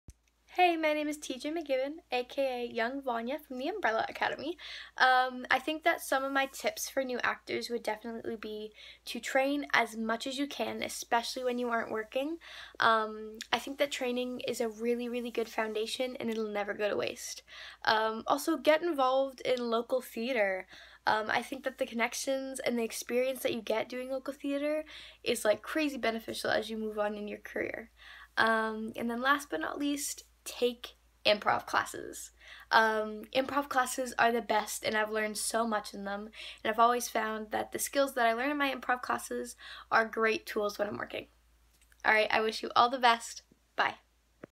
New Actor Advice from Umbrella Academy actress T J McGibbon young Vanya
Actress T J McGibbon plays young Vanya in Netflix's Umbrella Academy gives acting advice to new actors: training, local theatre, and improv!